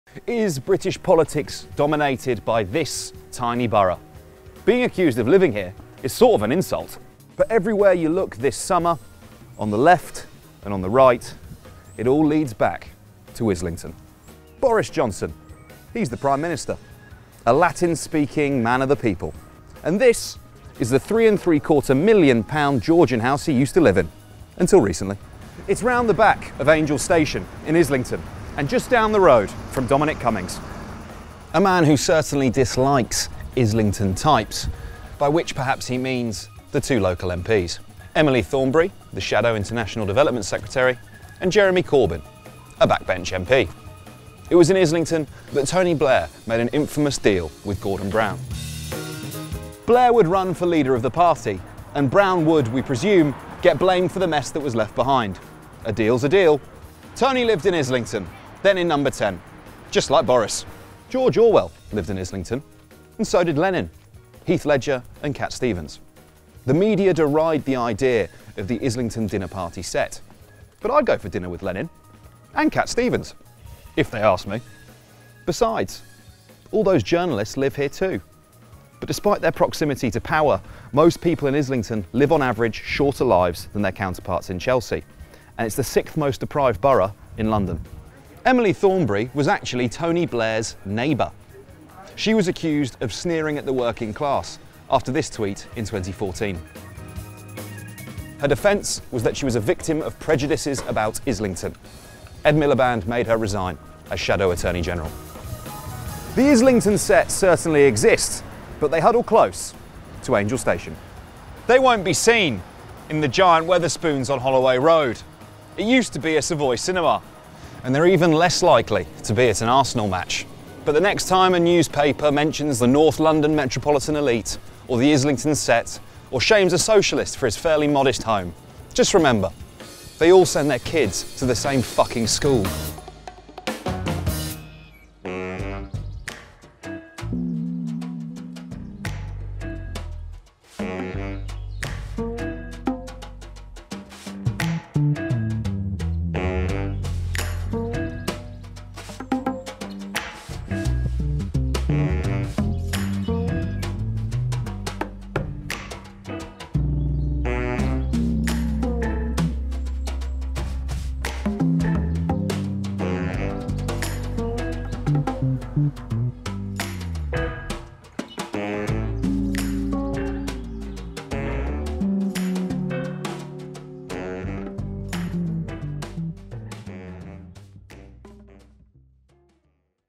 Islington: the prime London real estate that homes prime ministers
What do Tony Blair, Jeremy Corbyn, Boris Johnson and Dominic Cummings all have in common?

This tiny borough that everyone derides. Let’s talk about Islington.